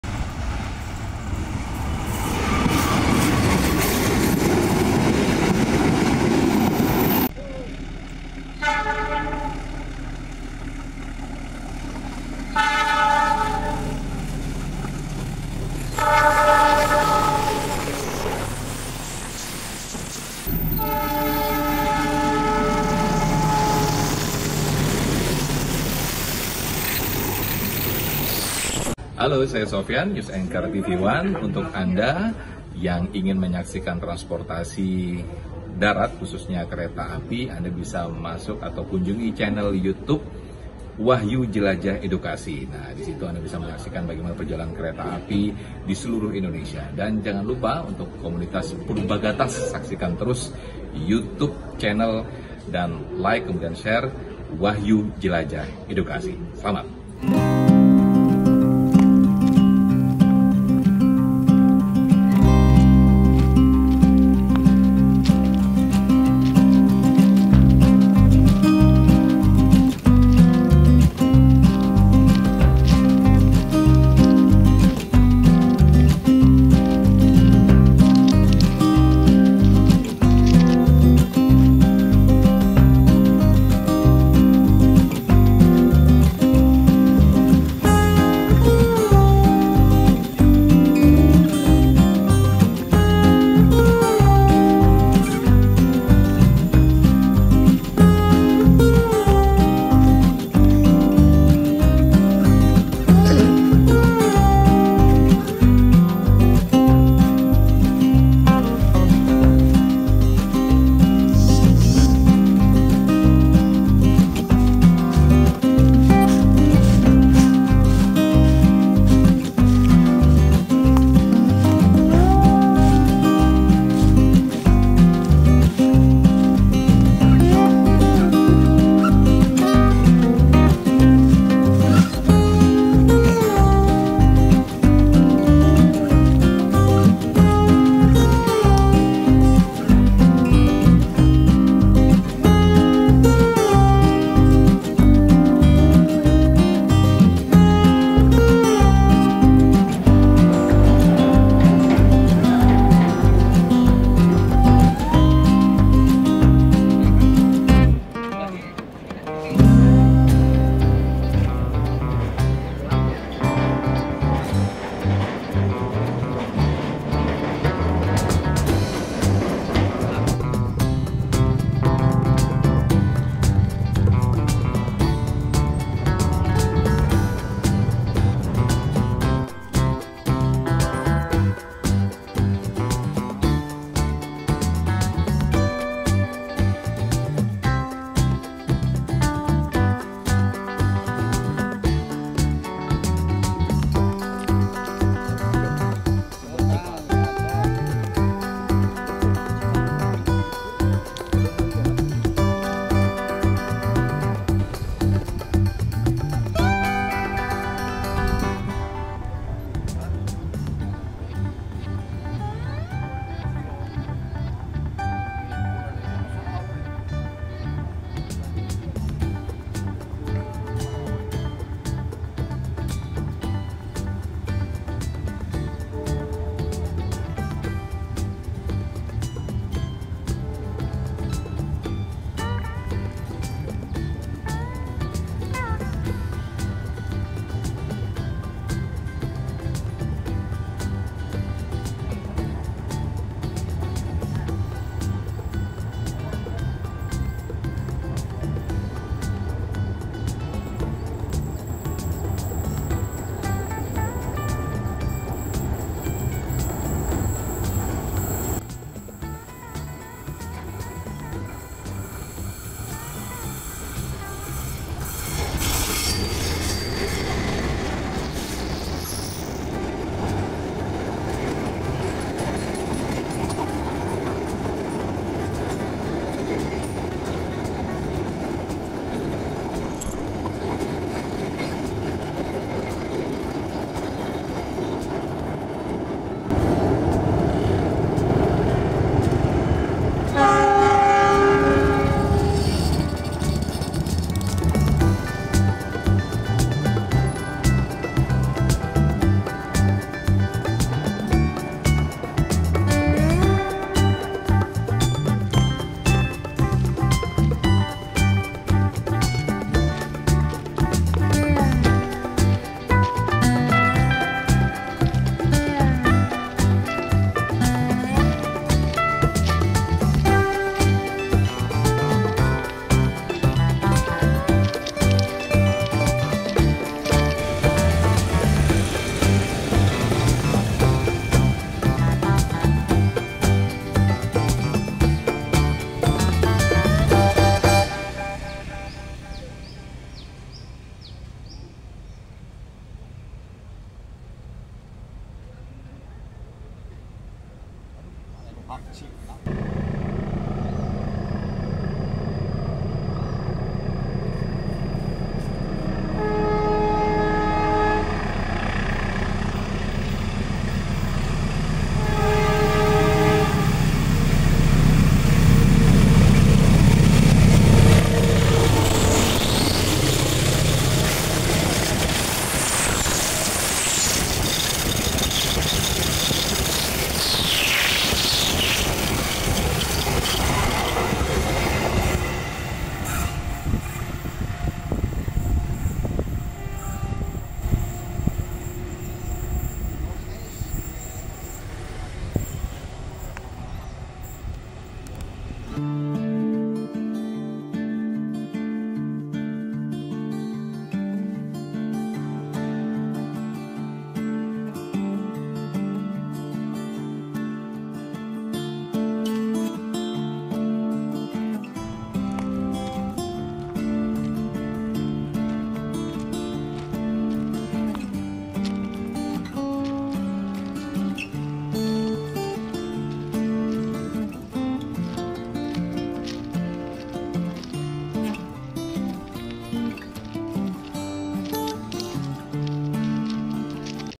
JEMBATAN KERETA CIRAHONG YANG EKSOTIS TERJAL DAN CURAM TRACKING NAIK TURUN BUKIT SAMBIL NONTON SEPUR
Kereta Dalam Video:
Kereta Kutojaya Selatan
Kereta Malabar

Lokasi Video:
Jembatan Kereta Api Cirahong Ciamis - Tasikmalaya